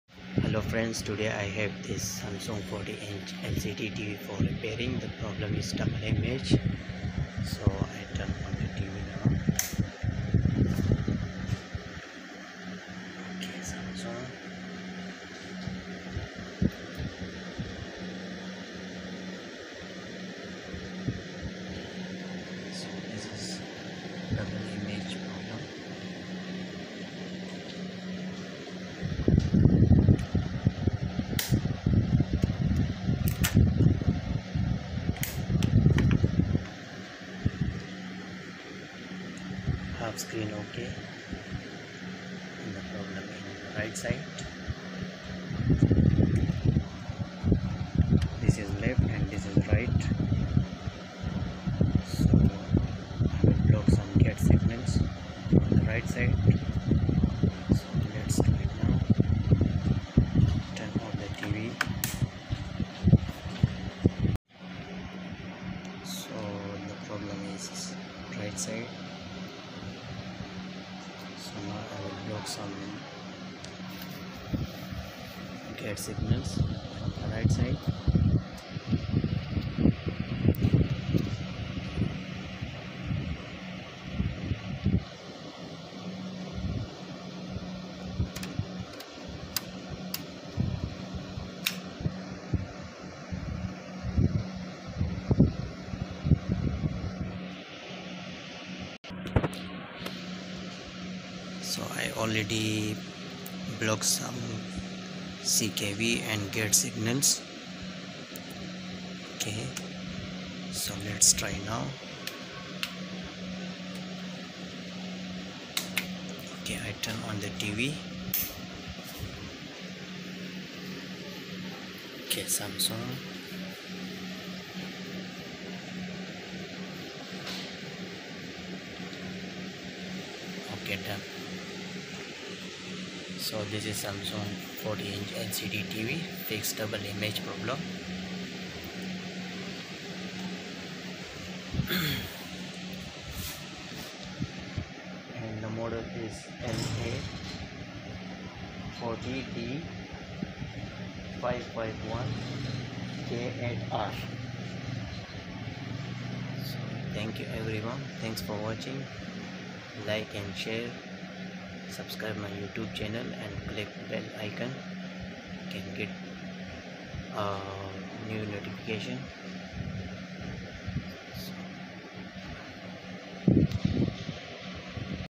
SAMSUNG 40 Inch LCD Tv Repair double image problem solve Model LA40D551K8R
SAMSUNG 40 INCH LCD TV
MODEL LA40D551K8R
REPAIR DOUBLE IMAGE PROBLEM SOLVE
REPAIR FROM T CON BOARD BLOCK SOME GATE SIGNALS